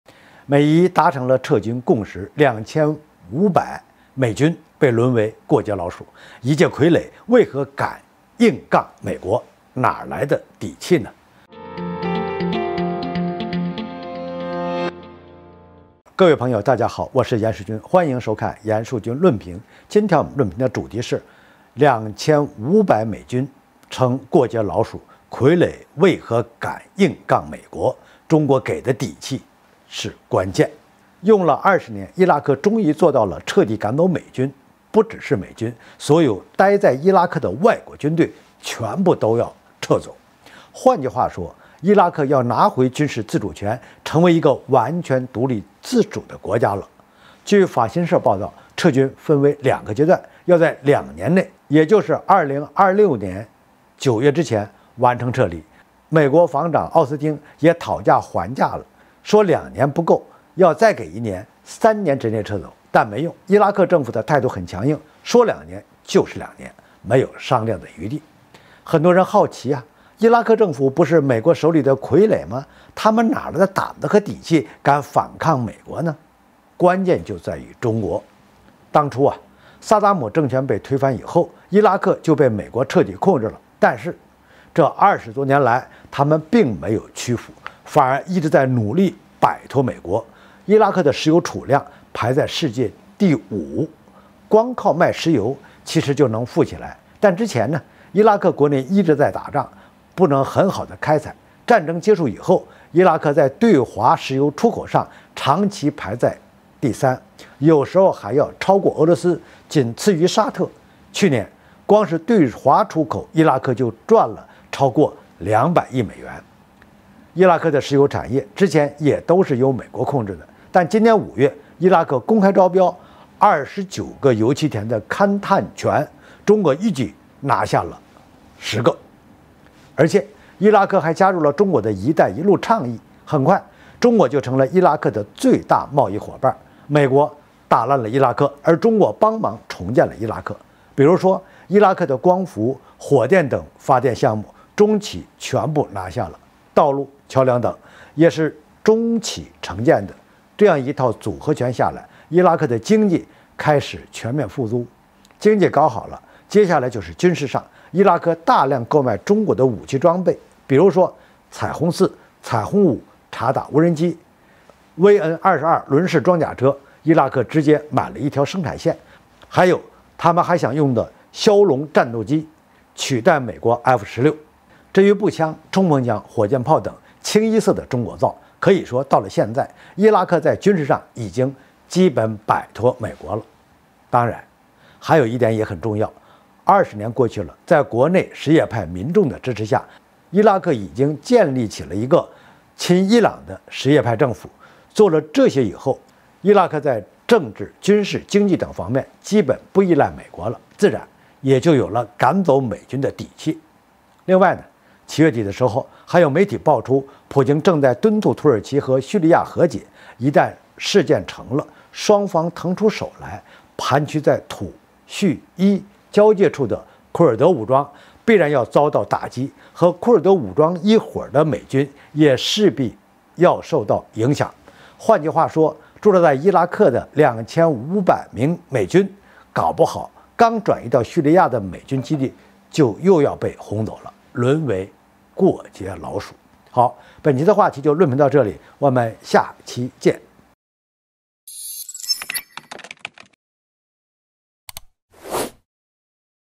2500美軍成“過街老鼠”，傀儡為何敢硬剛美國？中國給的底氣是關鍵
美伊達成了撤軍共識，2500美軍將淪為「過街老鼠」。一介傀儡，為何敢硬剛美國？哪來的底氣？